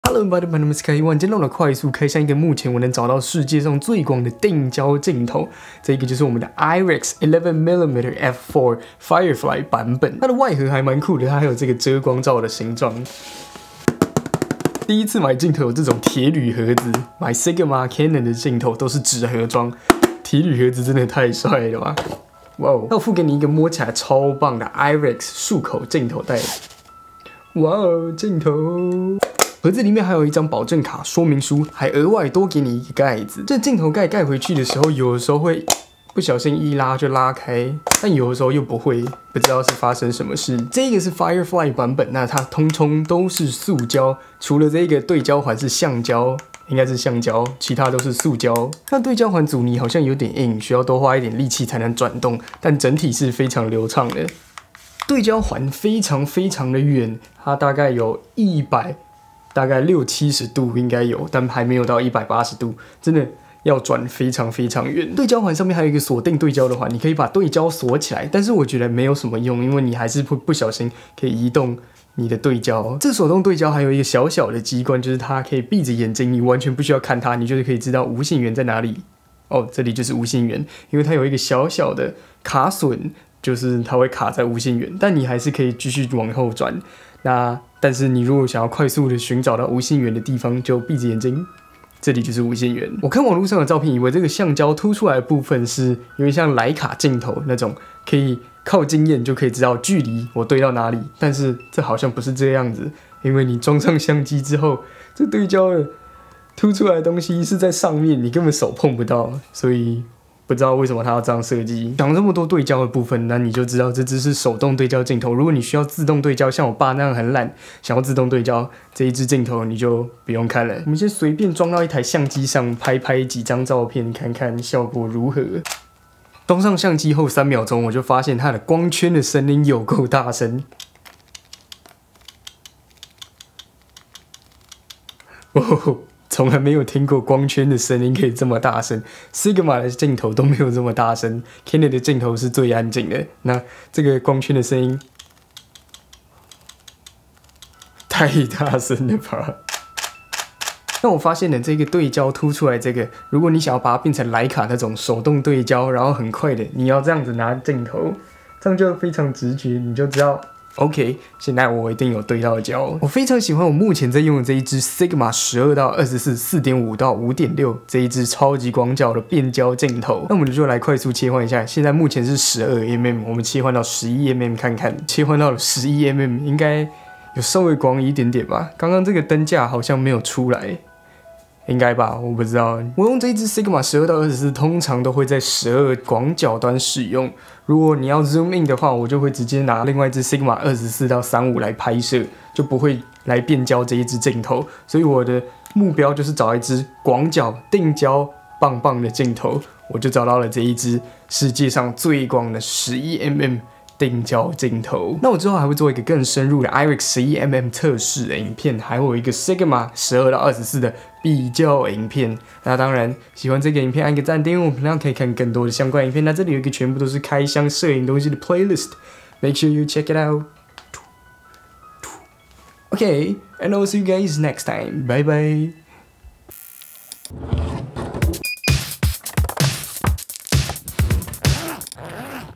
世界最廣的鏡頭 神鏡 平價廣角 定焦 快速開箱 外觀 操作 IRIX 11MM F/4.0 Firefly for canon
IRIX 11MM F/4.0 Firefly for canon
世界上最廣的鏡頭 神鏡 平價廣角 定焦 快速開箱 外觀 操作